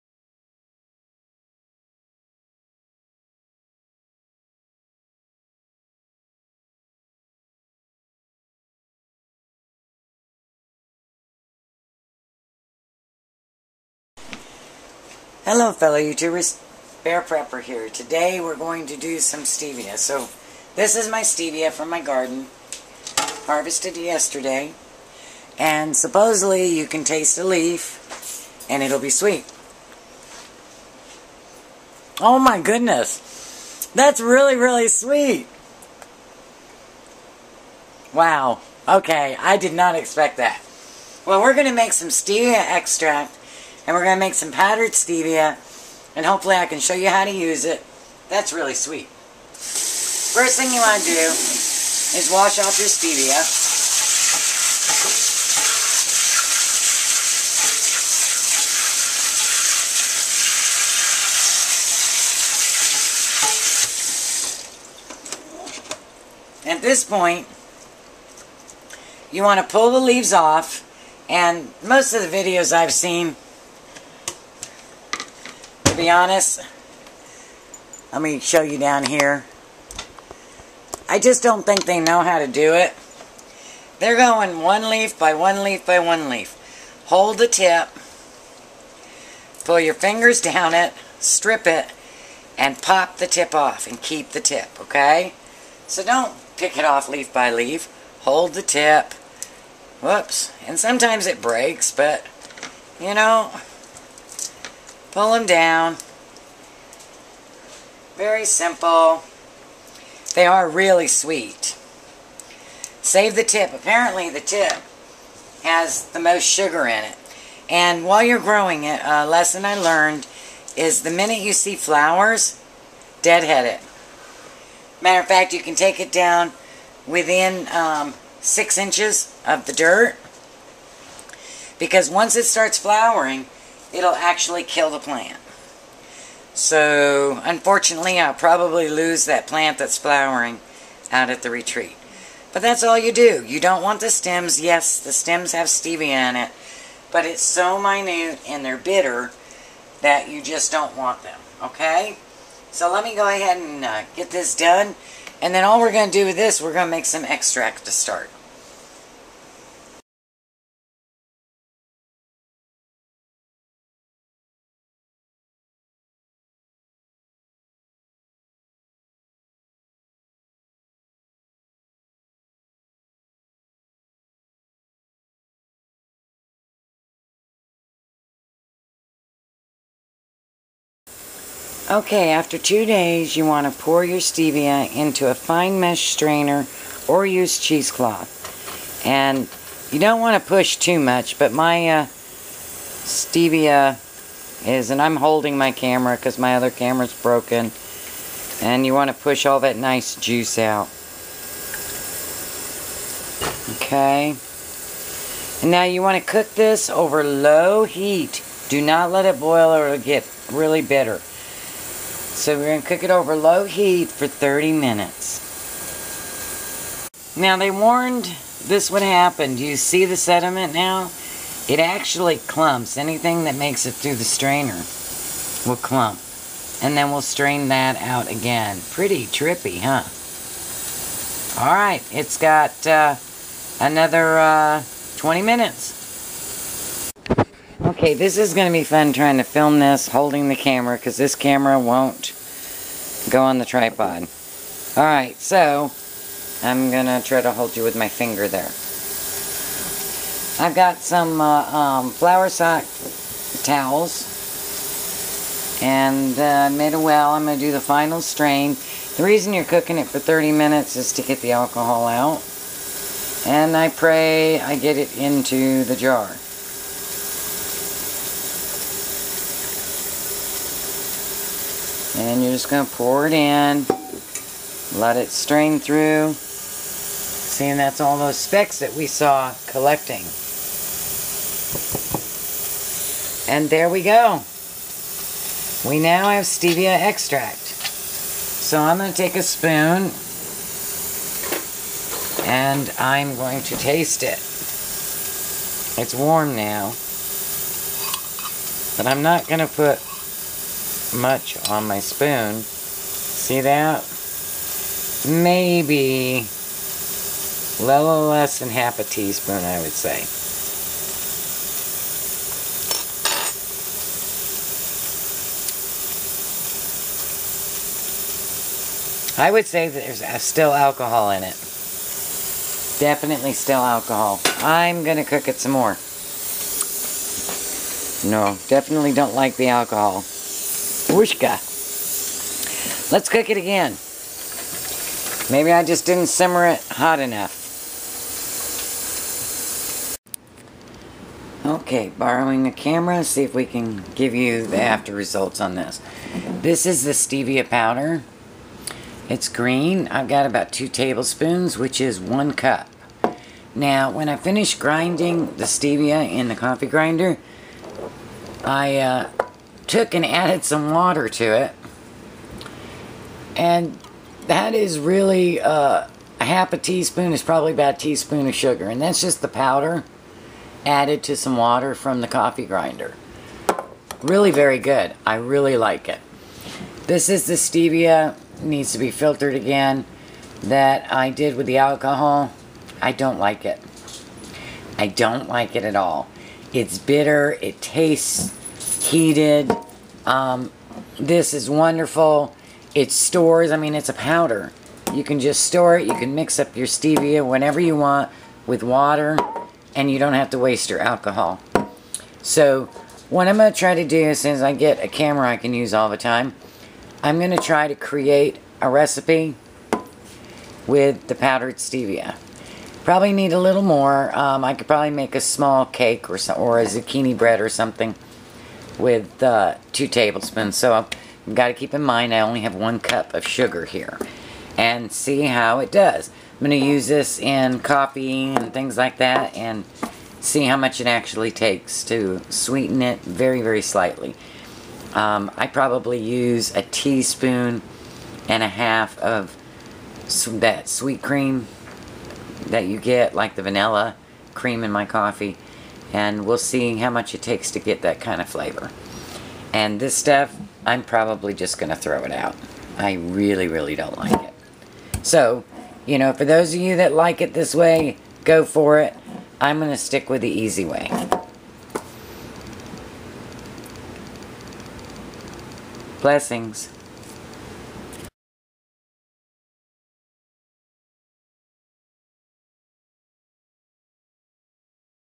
How to make sugar from your preppers retreat garden - Stevia
Growing Stevia is rather easy once you have a plant. I had no success growing from seed like most others. So watch your big box stores come spring and get you some Stevia at your retreat garden and you won't regret it. Blessings!
1st & 3rd Tuesday 8pm central time